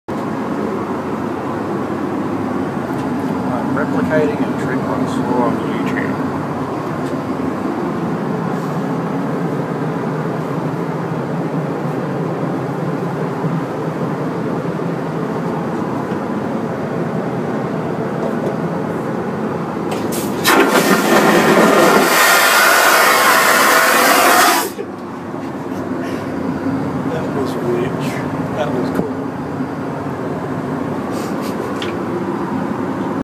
Airplane toilet trick
I did this on my trip to Darwin from Adelaide I was bored so I decided to try something I found on another video (I'm aware that the video is sideways if anyone knows how to fix it please tell me so I can re-upload it).Enjoy